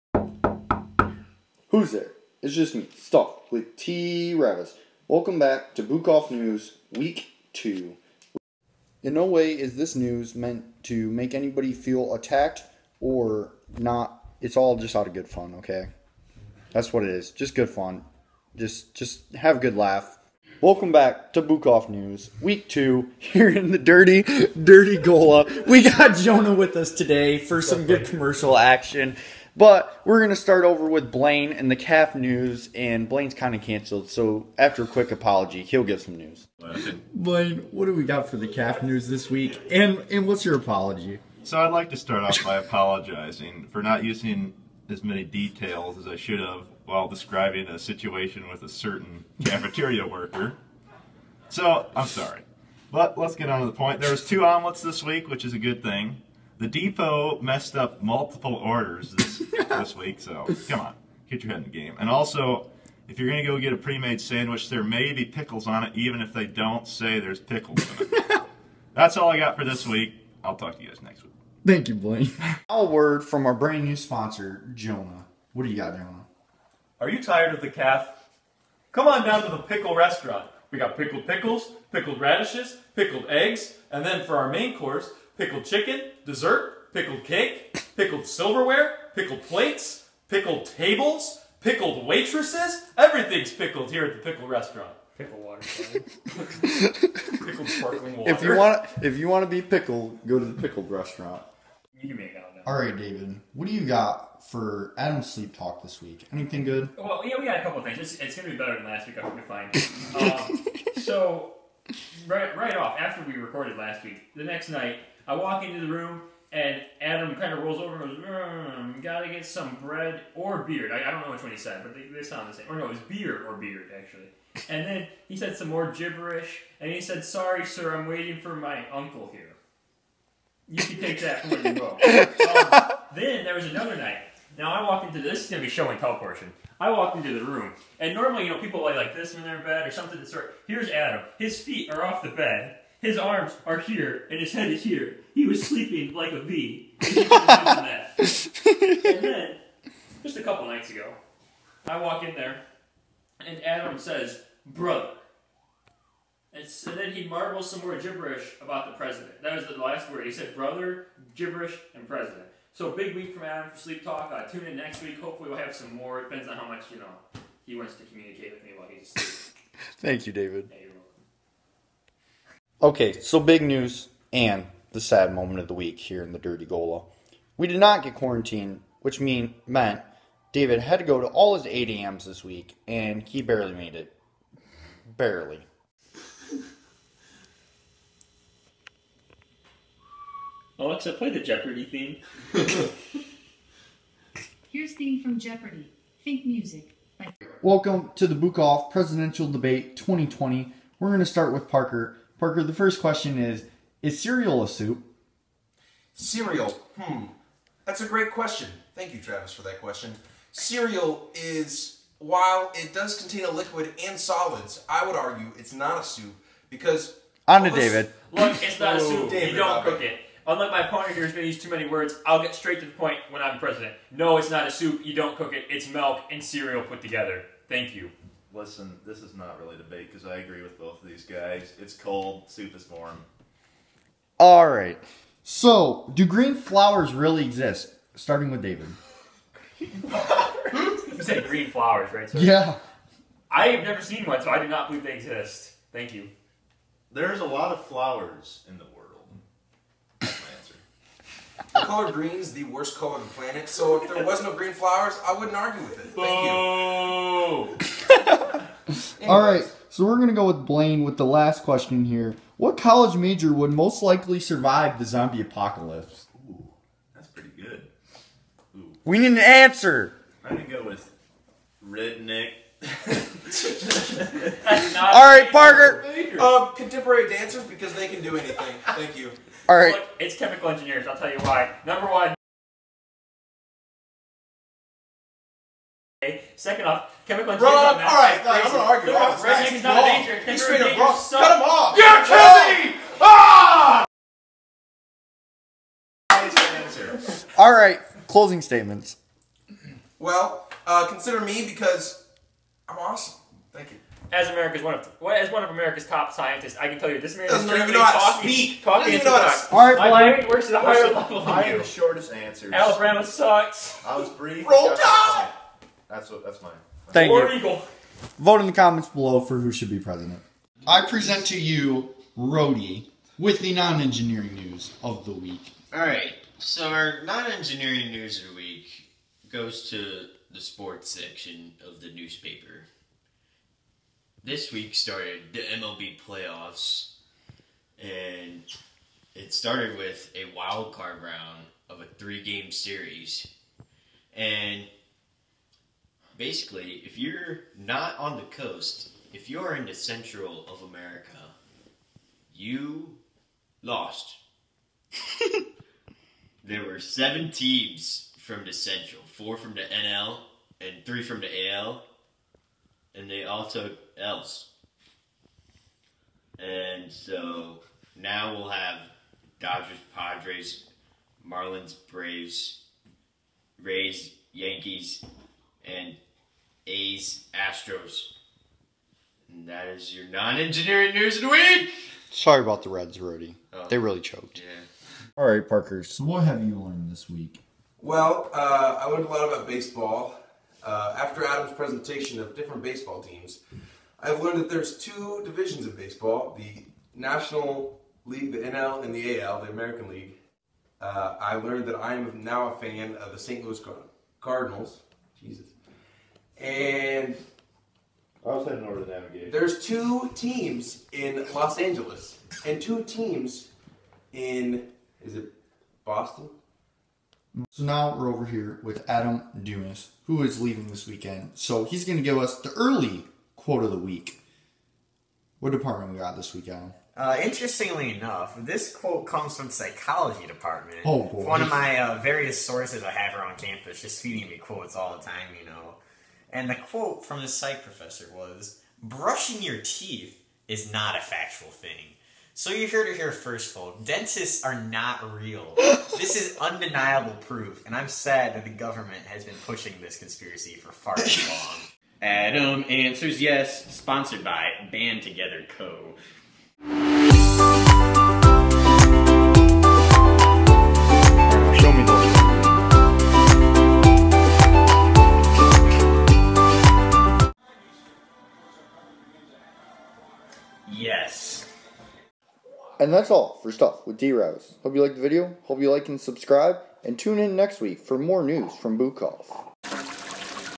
Bukoff News Week 2 (The Presidential Debate)
(A week in college vlog) Welcome to Bukoff News, crazy stuff that happens at college and the most reliable news station in Northeastern Indiana. This week my friends and I go over the stupid stuff that happened this week at college and the stupid stuff we did. In this episode, we have the best presidential debate that Trine University has ever seen.